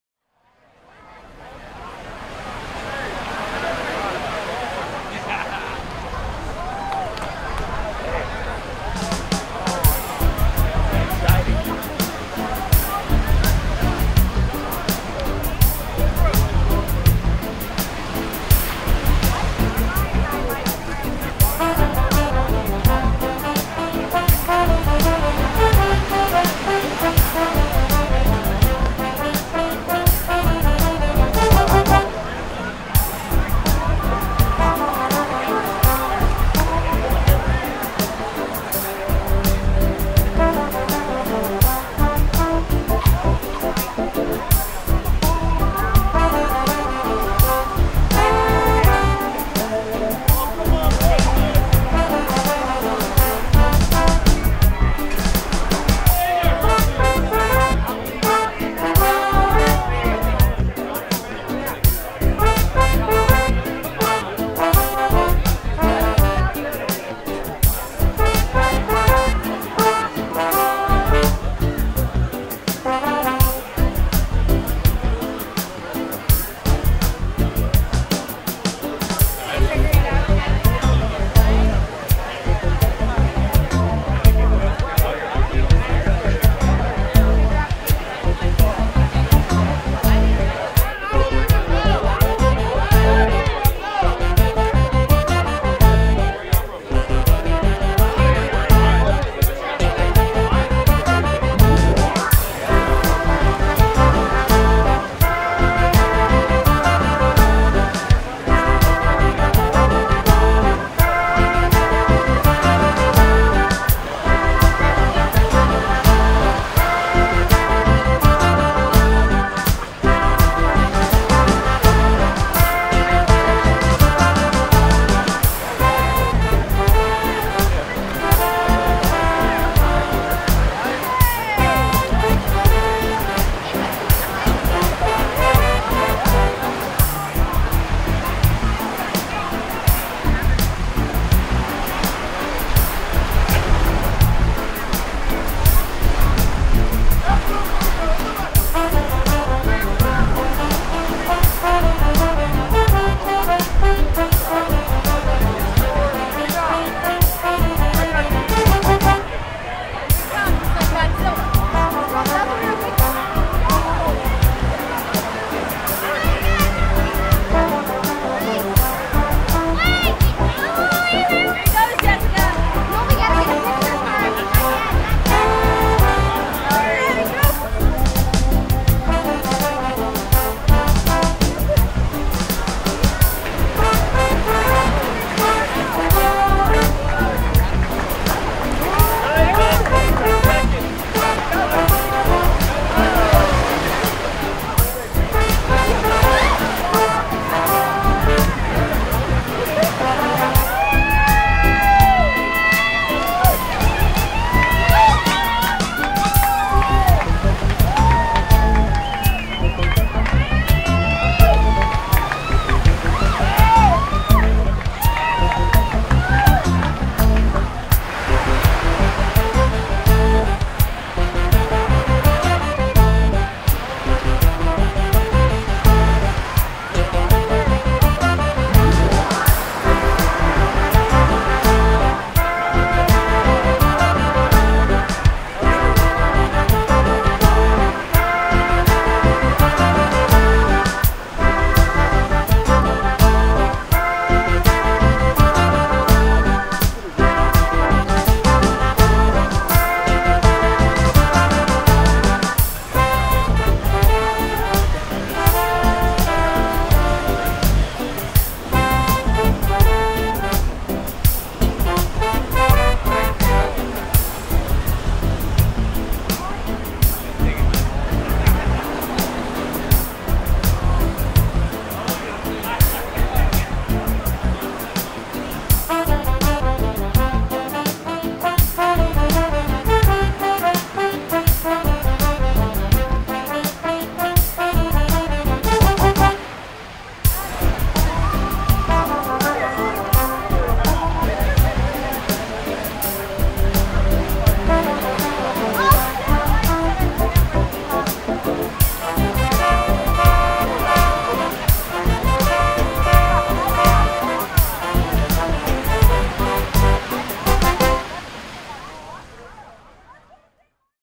British Virgin Islands - Soggy Dollar Beach Bar, White Bay, Jost Van Dyke, BVI, Caribbean
Now you can view 'Sophisticated Lady' BoatCam - updates live photo to website every 5 minutes during dayling hours at:

Join us for a spectacular Sunday of fun on the beach at White Bay, Jost Van Dyke in the BVI's!  Crowds abound, and a full harbor of boats make this the place to be for fun in the sun every Sunday at the Soggy Dollar Bar and One Love!  All natural sounds are intact, with a nice mellow reggae track playing in the background... Enjoy!
Filmed by Captain Rick
S/V 'Sophisticated Lady'